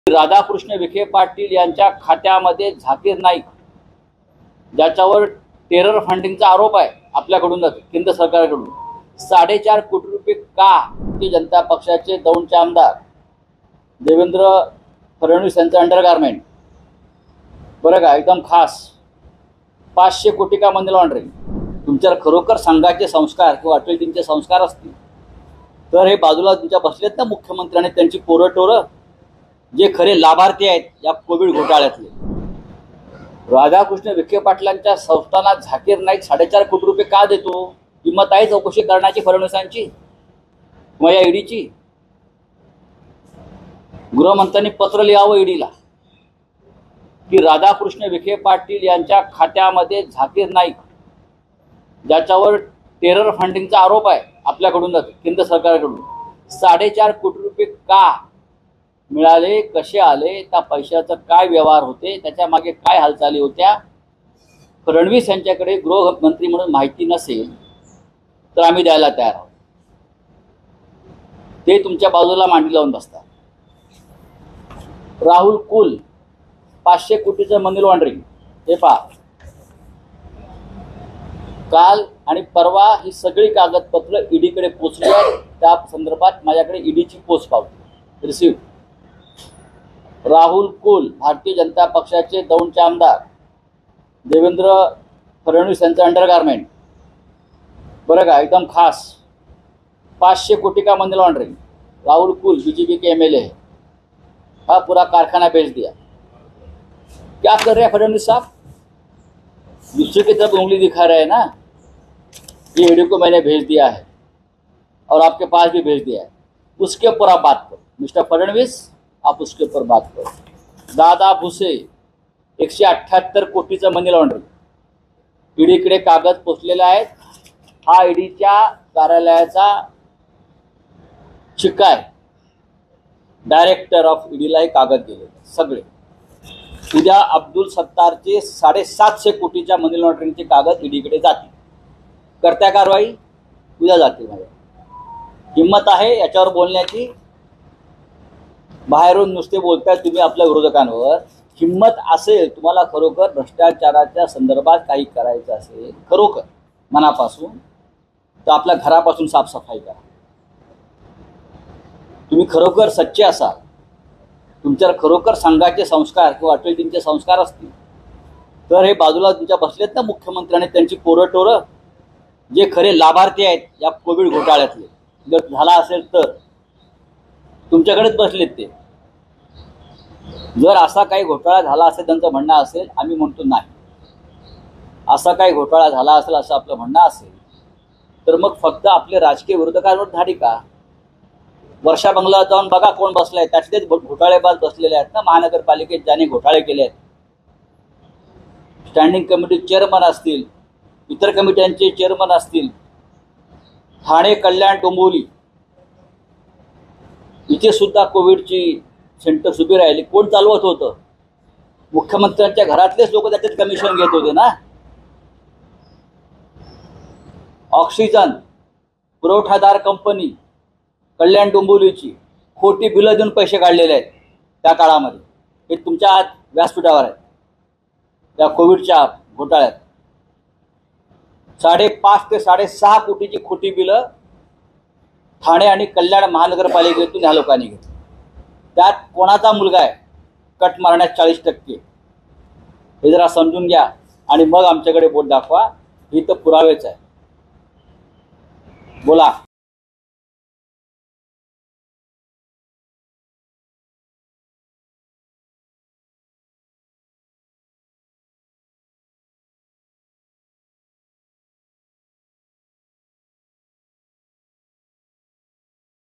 Sanjay Raut on Sujay Vikhe : विखे पाटलांच्या संस्थानात झाकीर नाईक साडेचार कोटी का देतो? | LetsUpp
गृहमंत्री देवेंद्र फडणवीस यांच्यावर खरच संघाचे संस्कार असतील, अटलजींचे विचार असतील तर त्यांनी त्यांच्या आजूबाजूला असलेल्या भ्रष्ट मंत्र्यांवर कारवाई करावी, असे आव्हान संजय राऊत यांनी फडणवीसांना दिले.